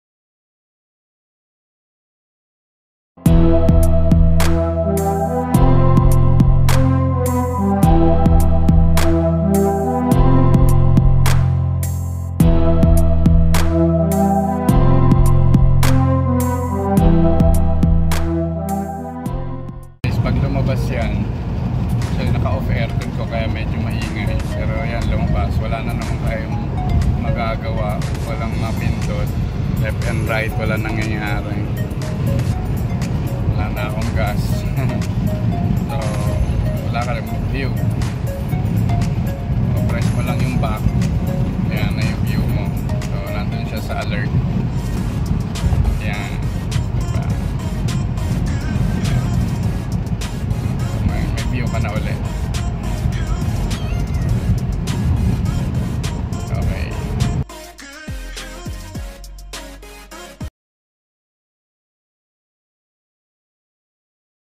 Veloz Tips: Low Fuel Warning
what happens during fuel low warning?
how to remove it?